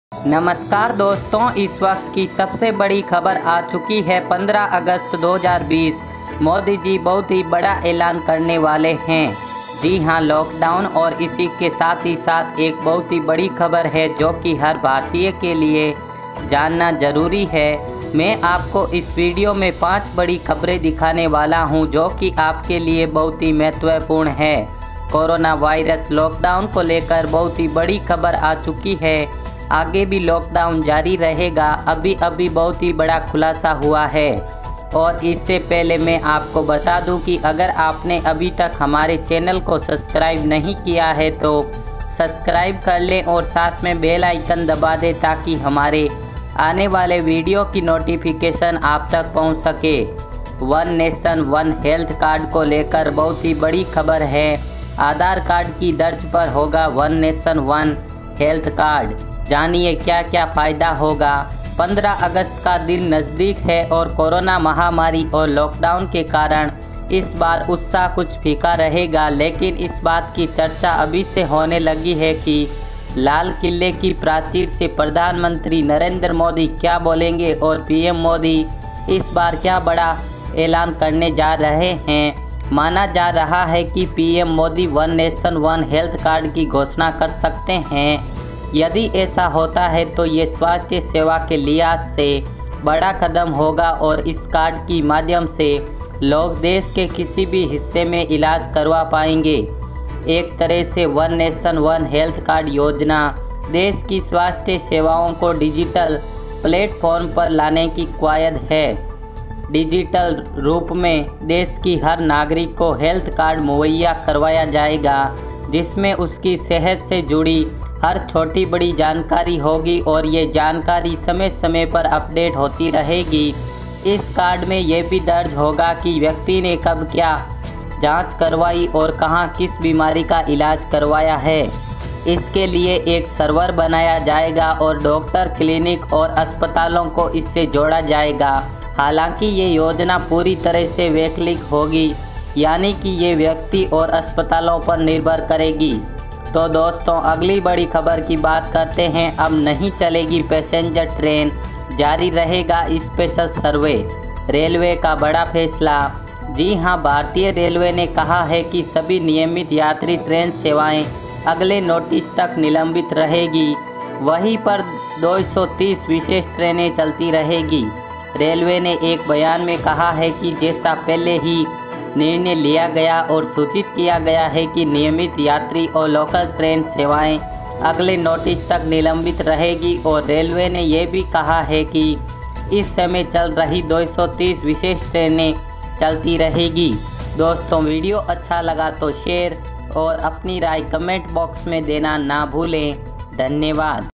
ब्रेकिंग न्यूज़ : पूरे देश में 17 सितंबर तक कर्फ्यू लॉकडाउन लागू,, पीएम मोदी ने किया ऐलान ! Lockdown
Subscribce to News 24 Rj Hindi channal for latest updates on moive and relete video...

and all breaking news video today.

News 24 Rj Hindi